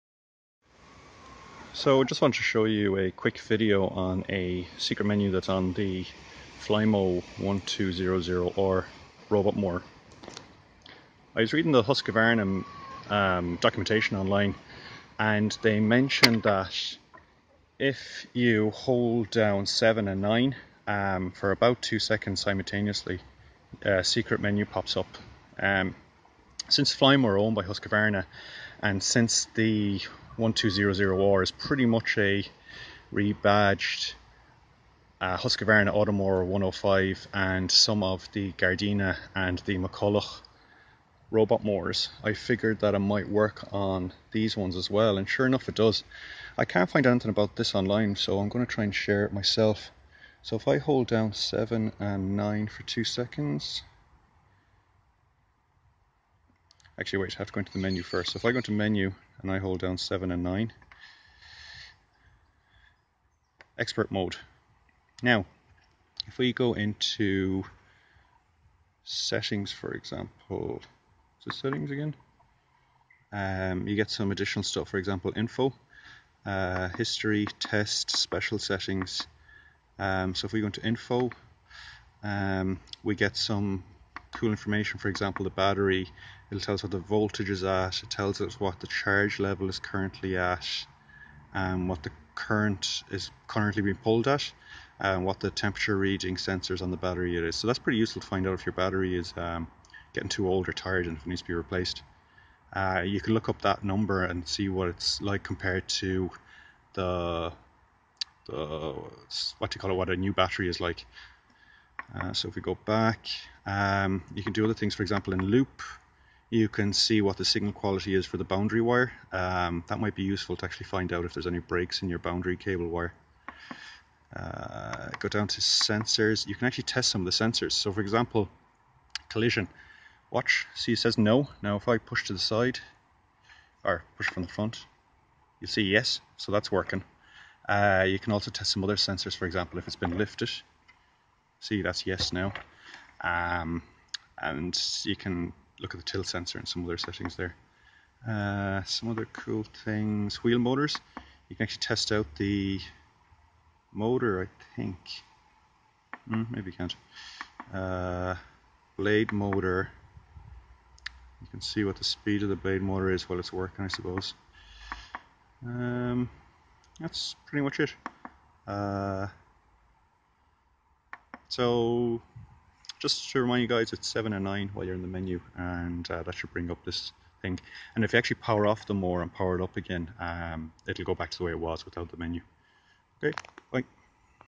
Secret "Expert mode" menu on Flymo 1200r
On the Flymo 1200r keypad, if you hold key 7 and 9 for ~2s you enable an "Expert" extra menu. This menu gives you the following features:
Estimated capacity of the battery in mAh. Very handy for knowing when your battery is getting old.
Sensor test menu. For example, you can test the tilt sensors, the bump sensors, etc
Motor test menu. For example, you can test the cutter motor, the wheel motors
Statistics menu
Boundary wire tests. Could be handy for finding breaks in the wire
And others that I cant remember

I would assume this extra feature will work with other rebadged Husqvarnas such as
Automower® 105
Gardena R38Li
Gardena R40Li
Gardena R45Li
Gardena R50Li
Gardena R70Li
Gardena R80Li
McCulloch ROB R600
McCulloch ROB R800
McCulloch ROB R1000
McCulloch Flymo 1200R